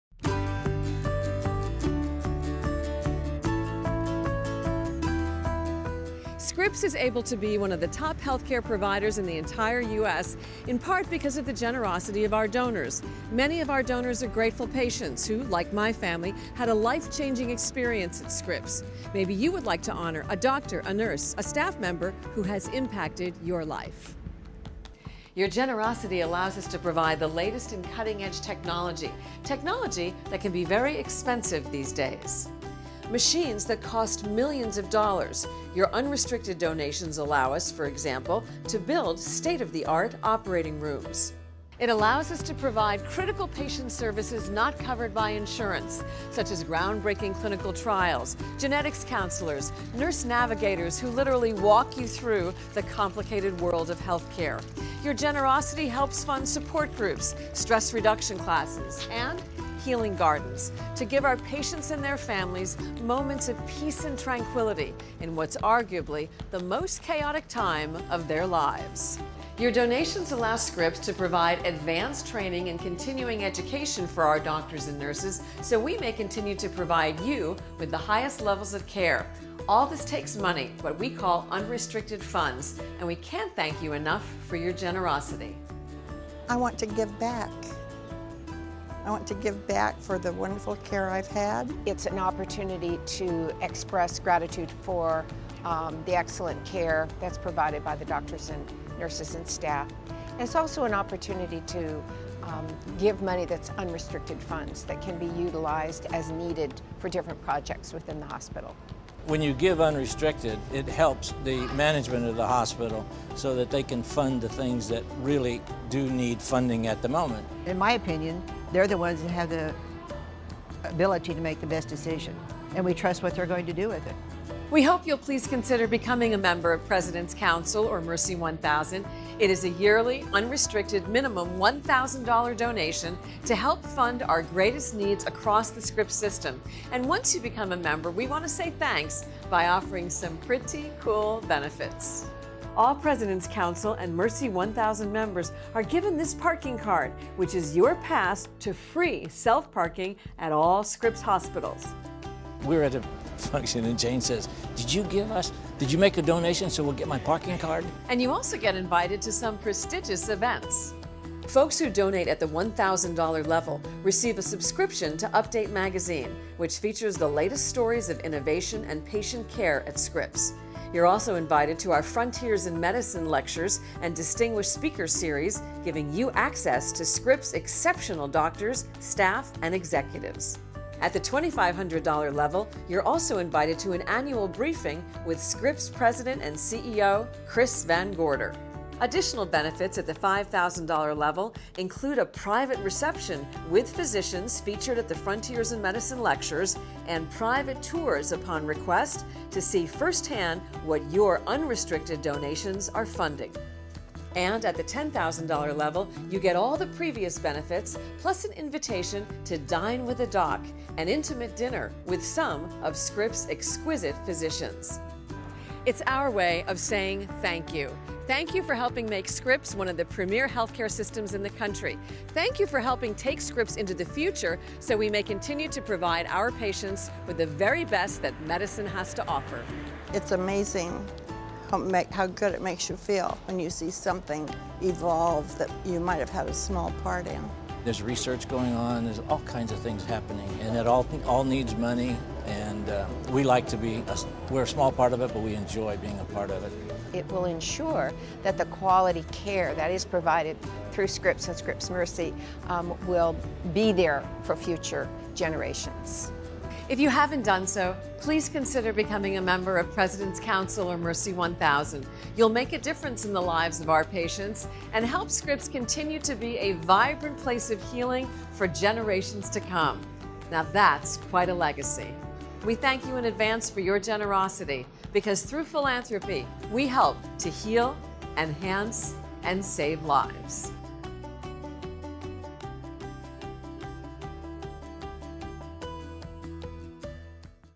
Why You Should Consider Joining Scripps President's Council
Learn more about Scripps President's Council and find out how joining could benefit you and your community.

Rated one of the top health systems in the U.S., Scripps Health provides leading-edge care made possible,  in part, through the generosity of donors. Scripps relies on philanthropy to help fund patient services not covered by insurance. It sustains our physician-researchers in their quest for breakthrough solutions. And it helps support the young physicians in our graduate medical education programs.

Susan Taylor, Scripps’ Executive Director of External Affairs, explains why a growing number of people, like you, are joining Scripps President’s Council and enjoying the exclusive opportunities that participation in the Scripps legacy offers.